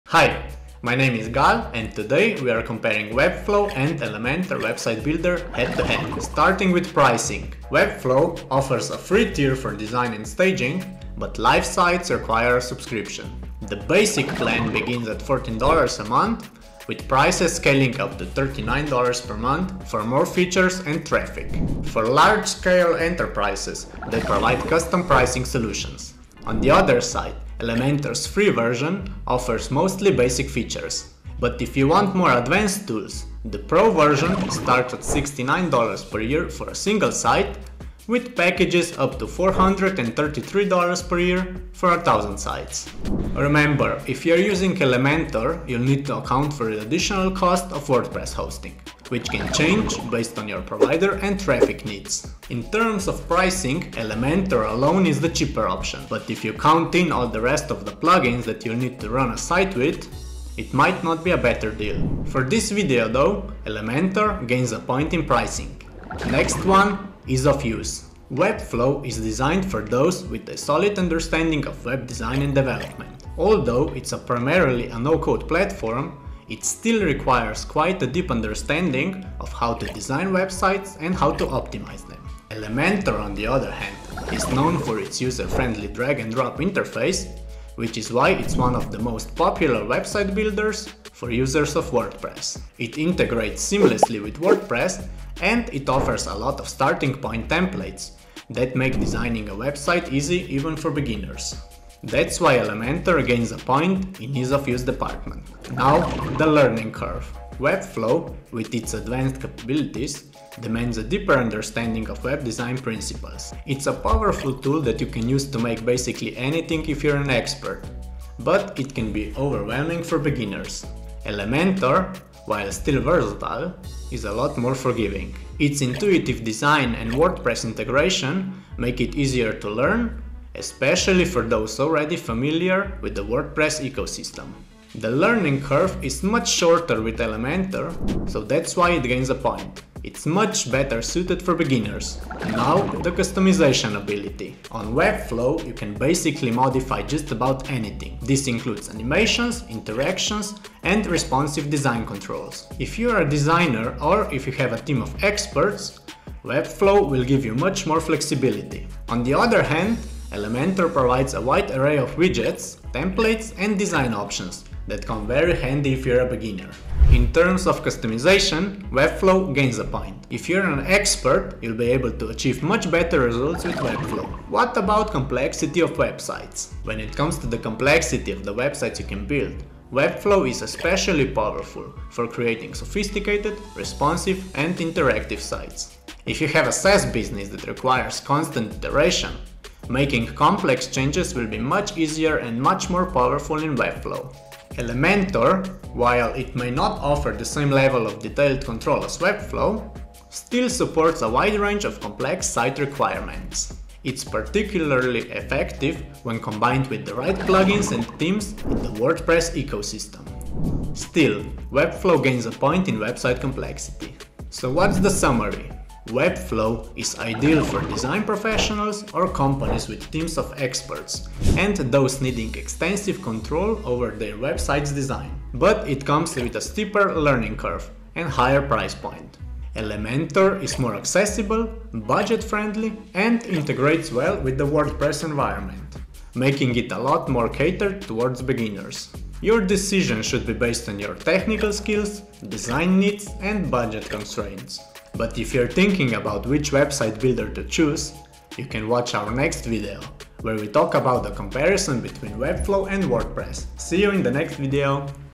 Webflow vs. Elementor - 2024 Updated Website Builder Comparison!
In this video Gal compares Webflow vs Elementor in almost 2 minutes :).

⏰ TimeStamps:
0:00 - Intro & Pricing
1:06 - Ease of use
1:44 - Learning Curve
2:23 - Customization
2:54 - Website Complexity

About:
In this video, Gal does a no-code website builder comparison between Webflow and WordPress plugin Elementor. In it he shows you which website builder to use in 2024 and how Elementor Pro vs Webflow compare in terms of pricing, ease of use and customizability.

If you are interested in website design and development tips, especially in Webflow follow us for more.